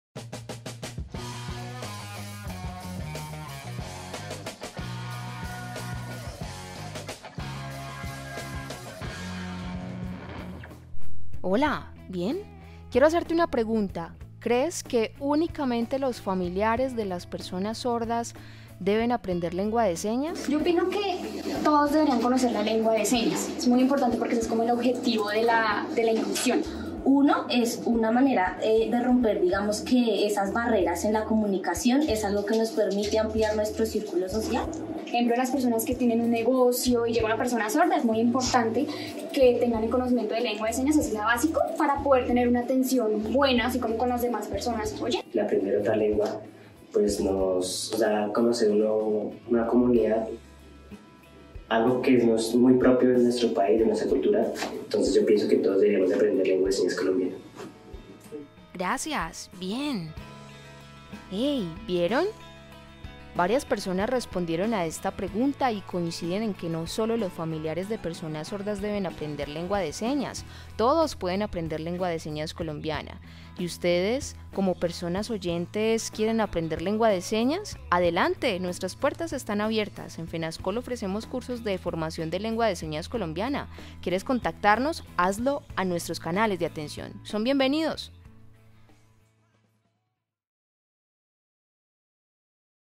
SCI FENASCOL - Con I de inquietudes preguntamos: ¿Quienes deberían aprender LSC?
SCI FENASCOL: una serie que te conecta con el aprendizaje de la LSC 😎

Con I de inquietudes preguntamos: ¿Quienes deberían aprender Lengua de Señas Colombiana- LSC?, ¿los encargados de los negocios comerciales y sus trabajadores?, ¿Los funcionarios públicos? ¿Qué otras personas deberían aprender LSC?

¡Claro! todas las personas deberían aprender esta lengua.